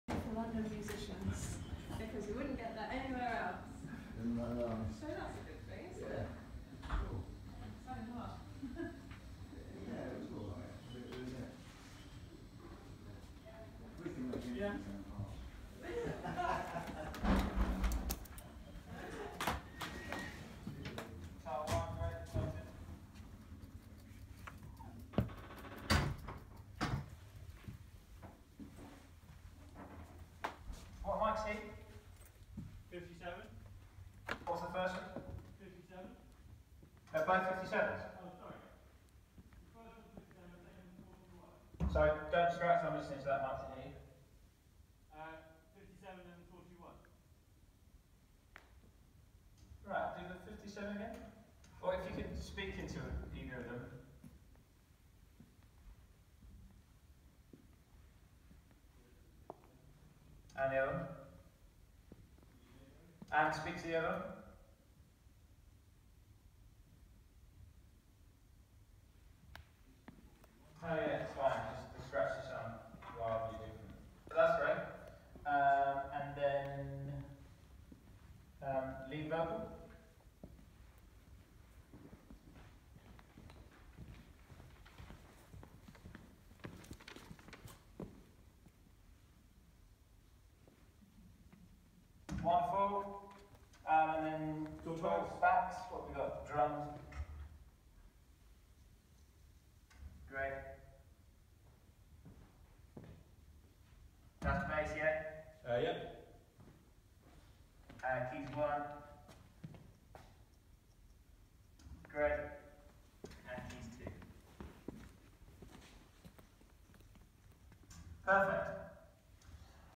Setting Up in Studio 2 at Abbey Road
Producer Sylvia Massy walks through the set-up and line check for the Stardragon session with the Abbey Road Institute in the historic Studio 2 at Abbey Road in London.